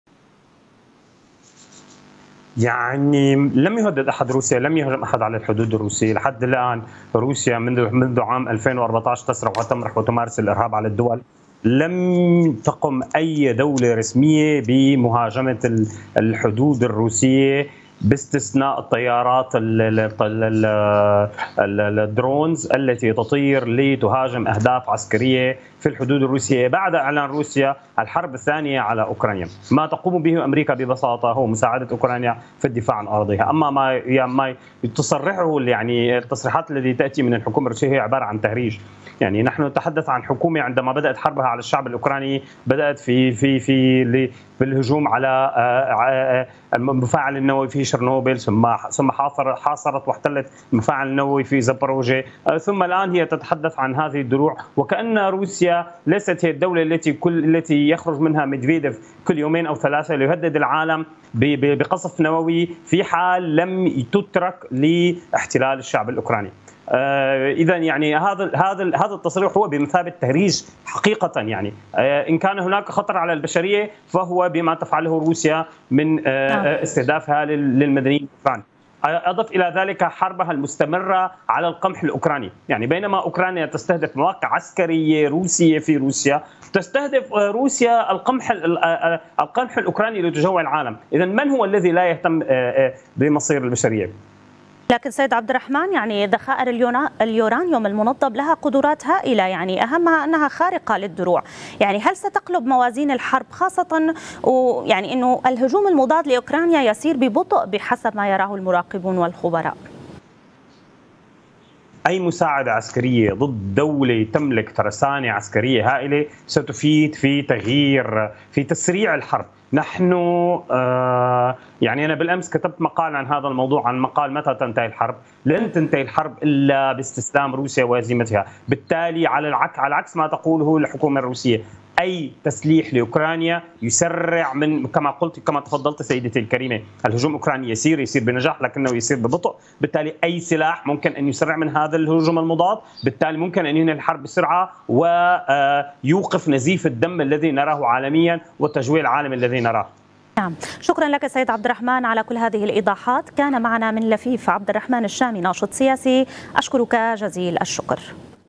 عبد الرحمن الشامي:أي مساعدة عسكرية غربية لا سيما من الولايات المتحدة ستساعد أوكرانيا في صد ...
لفيف
عبد الرحمن الشامي - ناشط سياسي

أي مساعدة عسكرية غربية لا سيما من الولايات المتحدة ستساعد أوكرانيا في صد الهجوم الروسي.
البث الحي / البث المباشر الأعلى مشاهدة

تابعوا قناة اليوم على التردد:
12687_27500 (H) _NILESAT